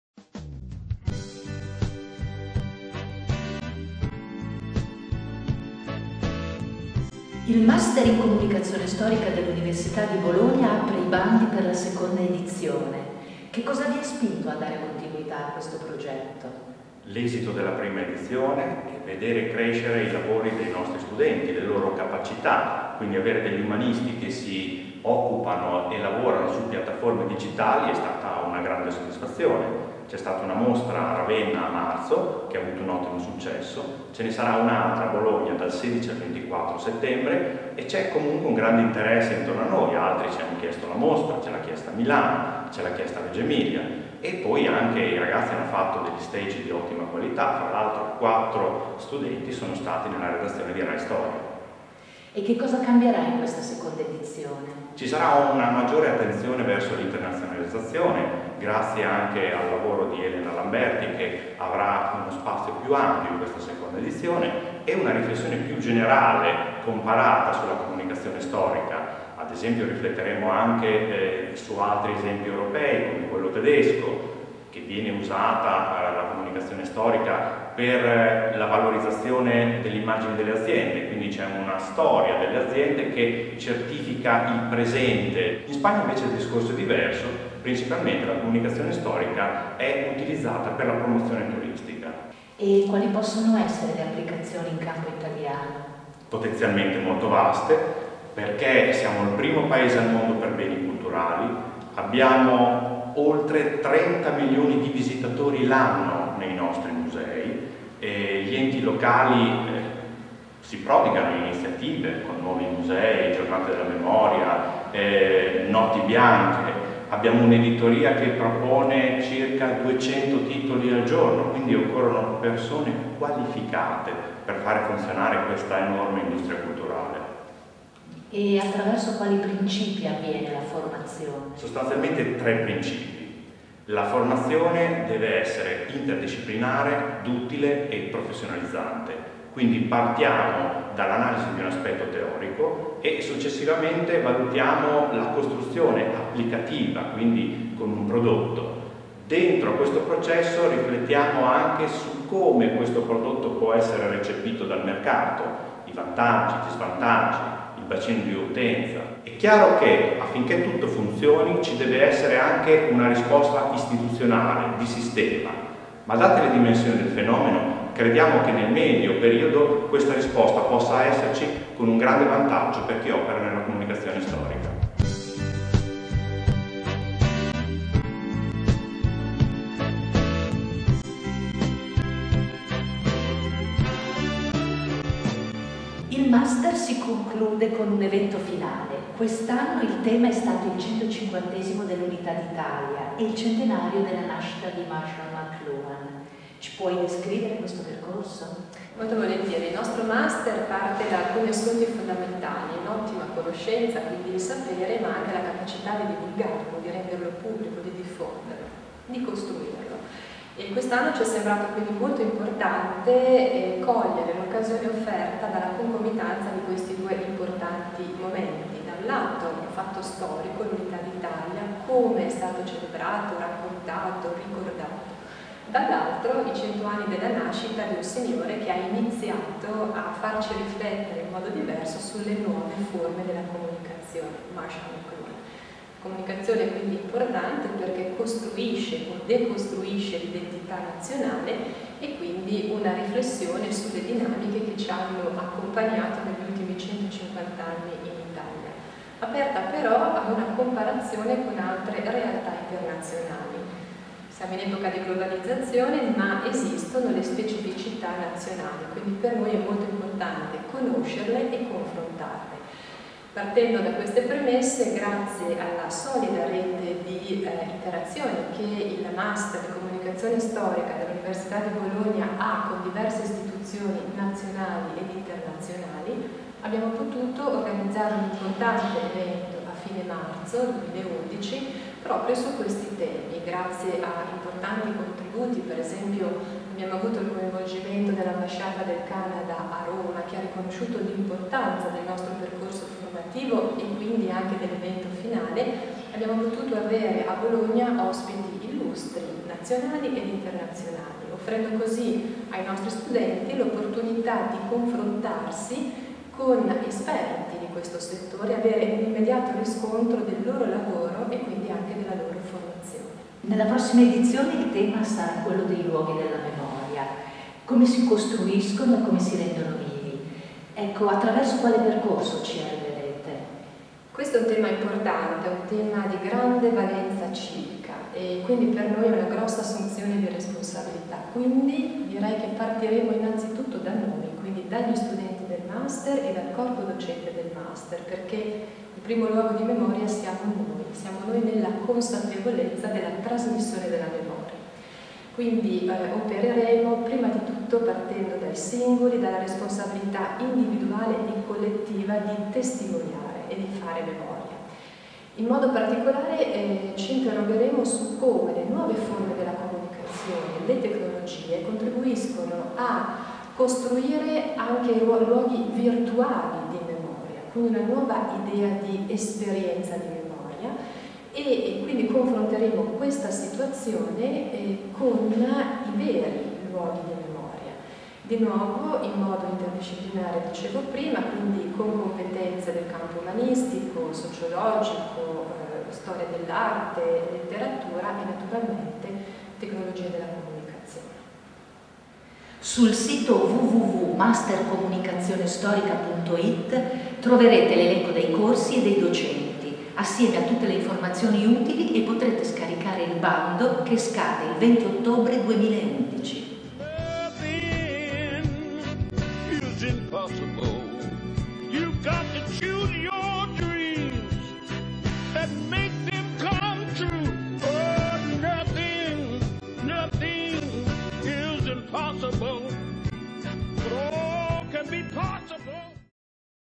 Master in Comunicazione Storica ed. 2011/2012
Il video di presentazione Master in Comunicazione Storica, Università di Bologna, edizione 2011/2012.